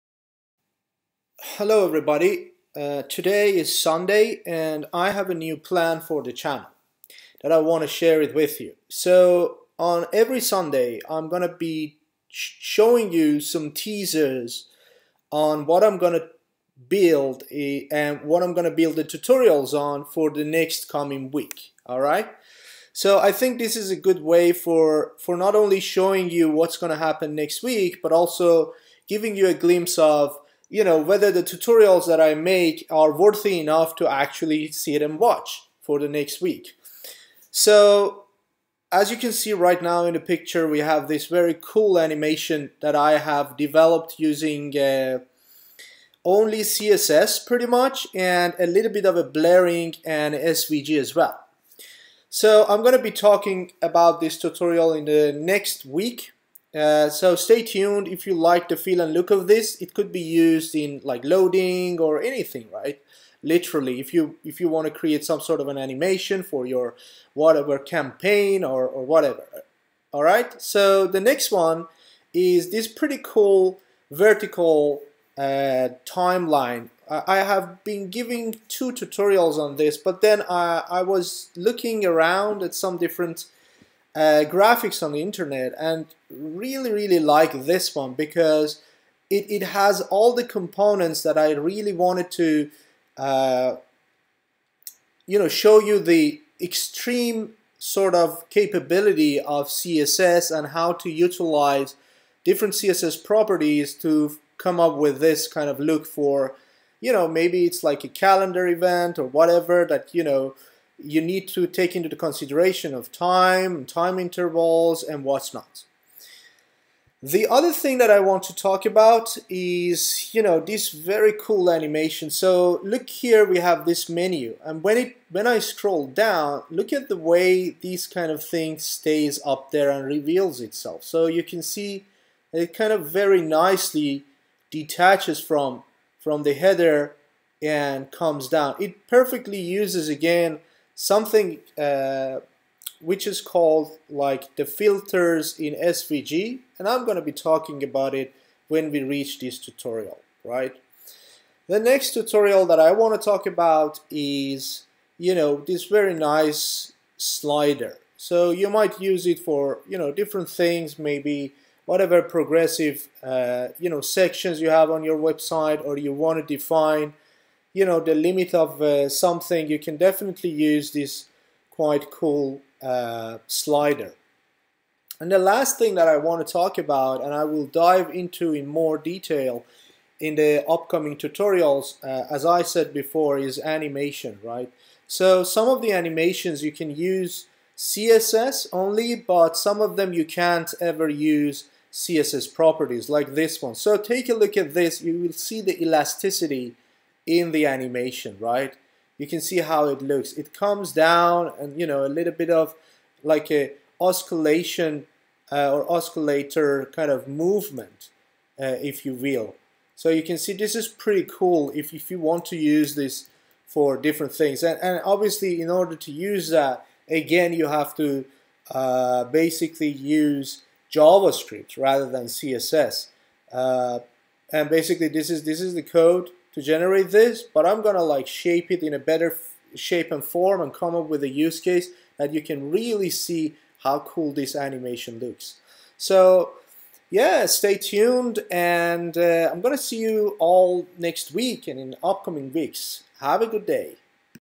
[Teaser] Css animation, SVG, SVG Filters, Css only vertical timeline, Elastic movements
This is a glimpse on tutorials for the upcoming week. Following is the list:
 - Vertical timeline
 - SVG Filters
 - CSS animations
 - Cool slider
 - Elastic timing functions for Elastic movements